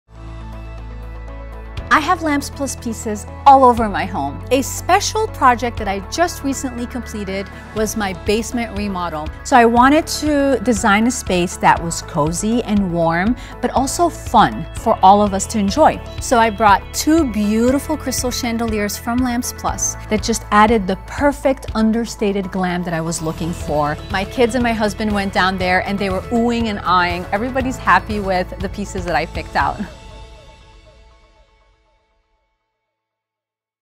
Farah Merhi of Inspire Me! Home Decor Shows Her Style with Lamps Plus
Farah Merhi of Inspire Me! Home Decor shows us how she uses Lamps Plus to find pieces that match her traditional, yet luxe style.

VIDEO OVERVIEW

Based in Grand Rapids, Mich., Farah is one of the Lamps Plus design ambassadors for 2019. She is the founder and CEO of the Inspire Me! Home Decor brand, and is one of the top influencers in her category.  Farah is the creator of the Instagram channel Inspire Me! Home Decor, which she launched in 2012 and now has millions of followers.

Recently, Farah did a complete remodel in her basement and she put in two crystal chandeliers from Lamps Plus to add a fun element to the warm space.